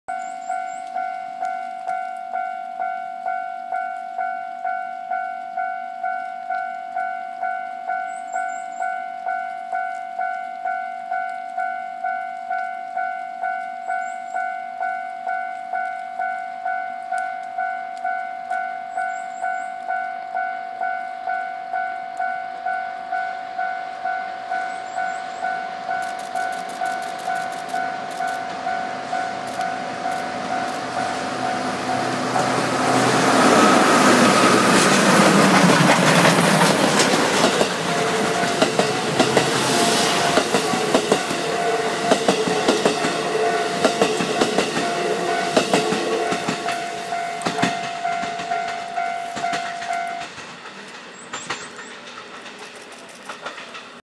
【爆煙DD】D51200不具合のためDD51が先頭で走るSLやまぐち号
2018年4月7日、仁保駅で発生したコンプレッサー不具合のため、津和野→新山口のSLやまぐち号は、DD51が先頭を走ります。
DD51-D51-35系の順番です。
全国各地の名所やイベント、鉄道関連の動画をアップしています。